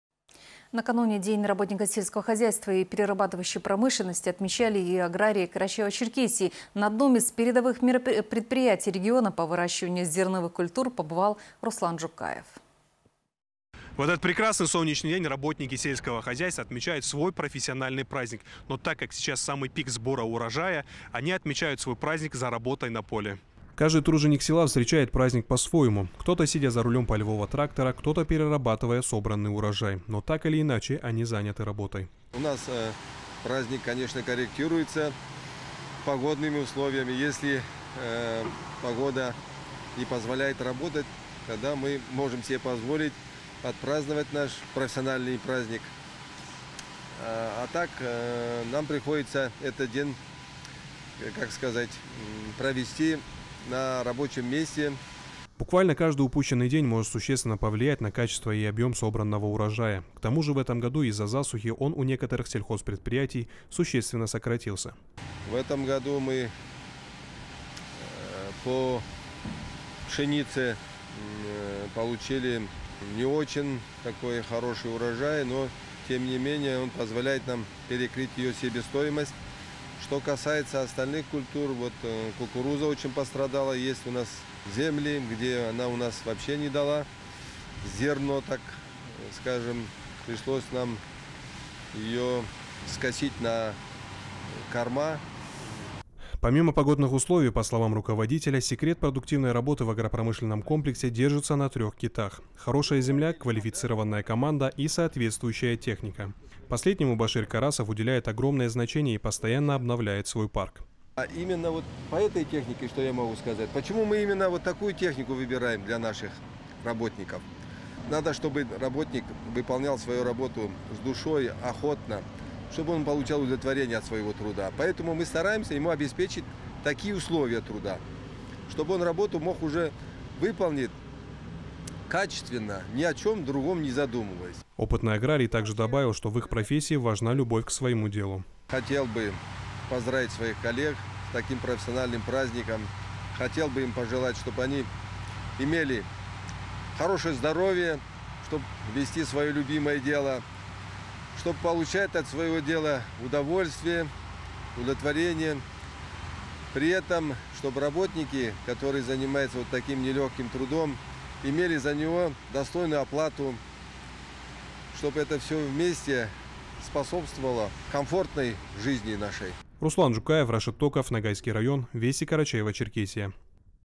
День работника сельского хозяйства и перерабатывающей промышленности отметили аграрии КЧР
День работника сельского хозяйства и перерабатывающей промышленности отметили аграрии Карачаево-Черкесии